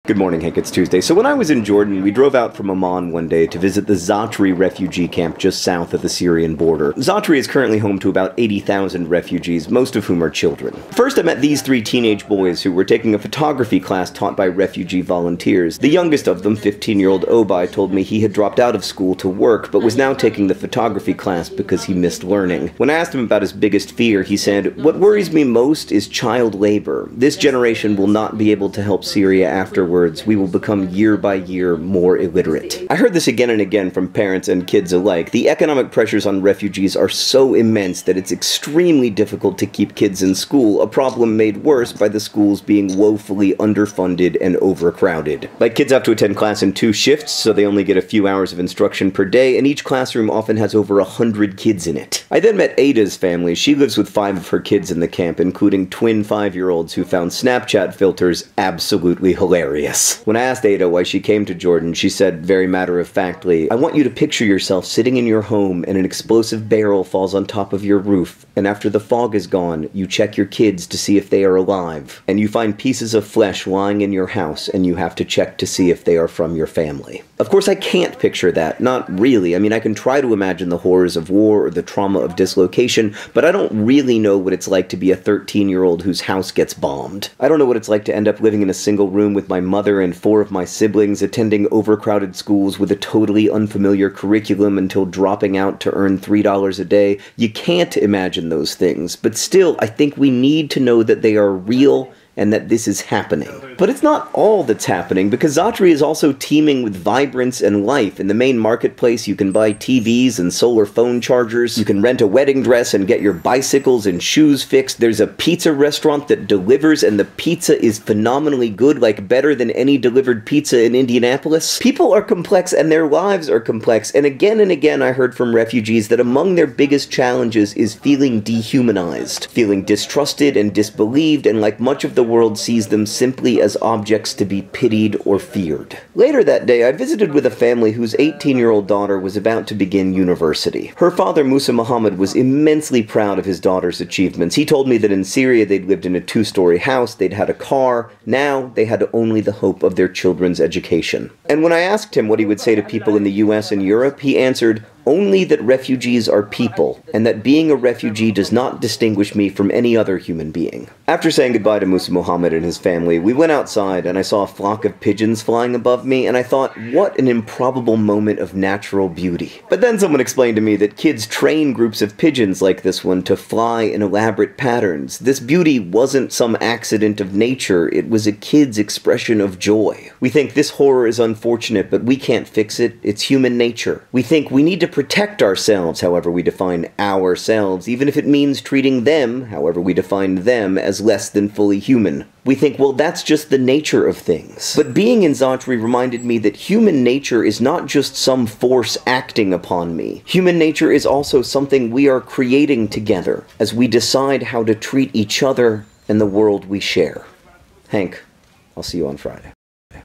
Zaatari: Thoughts from a Refugee Camp
In which John visits the Za'atari refugee camp in northern Jordan and talks with refugees there about their hopes, ambitions, fears, and complex lives.
Thanks to everyone at UNHCR, the UN's Refugee Agency, for allowing me to visit Zaatari and for all the work they do.